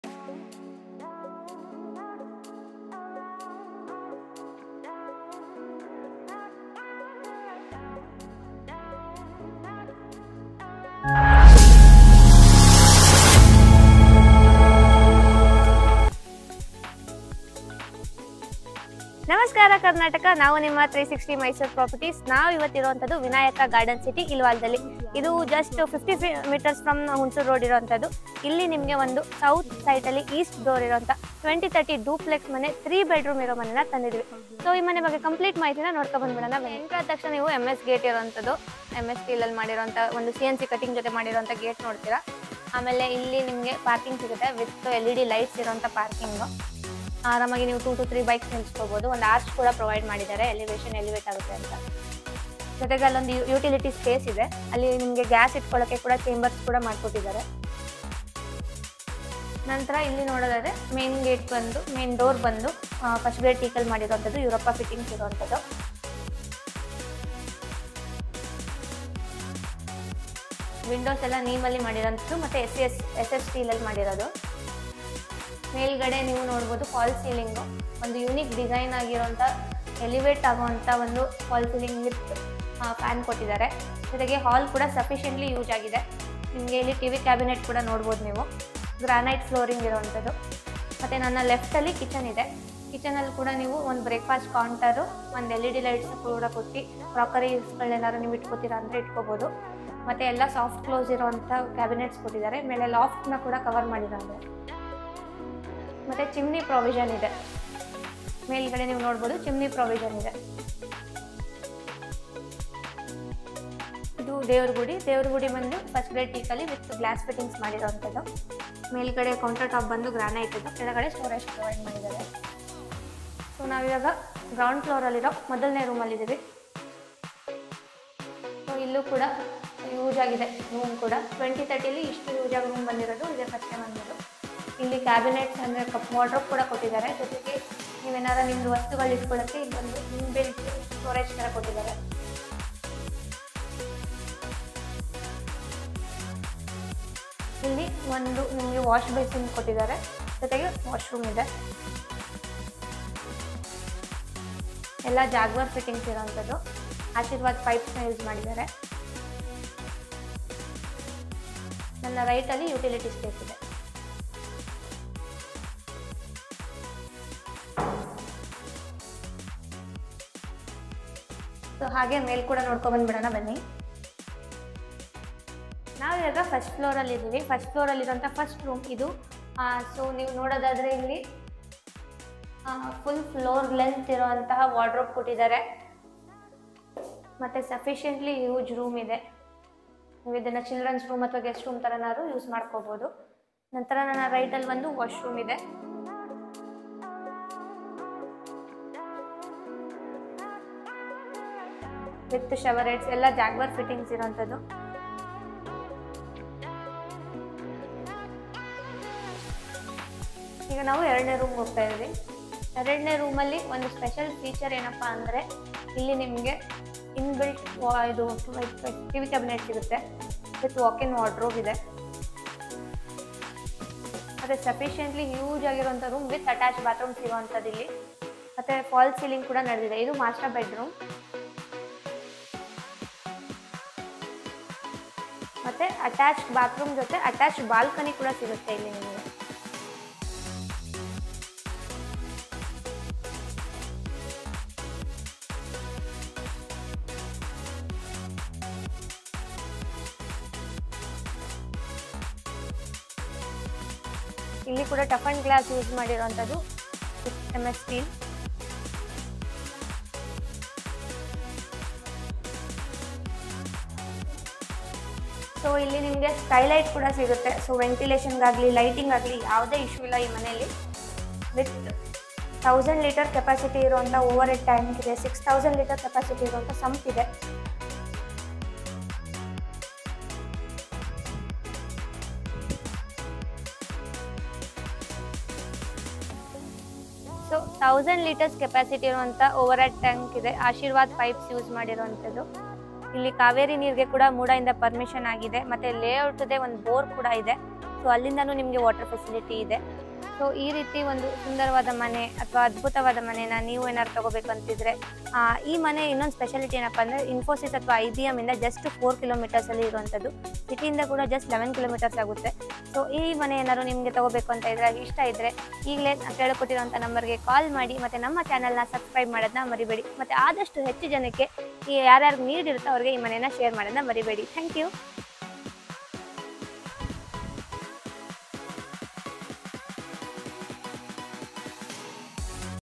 20X30 DUPLEX HOUSE FOR SALE ,3BHK, S/E FACE ,11kms from city
20x30 house for sale at Vinayaka garden city ,Mysore
3BHK
6000ltrs sump
1000ltrs over head tank
south face site east face door
parking space for 2 bikes
LED lights
kaveri water provision
Ashirvad pipes
Italian modular kitchen
semi furnished
teak wood  for main door and pooja door
granite flooring
jaguar fittings
lisha switches
V guard electricals
skylight
100% vastu
upto 80% loan

*Amount mentioned in the video is only the downpayment. Remaining amount can be arranged through Bank Loan provided customer satisfies all the loan eligibility criteria

3BHK
6000ltrs sump
1000ltrs over head tank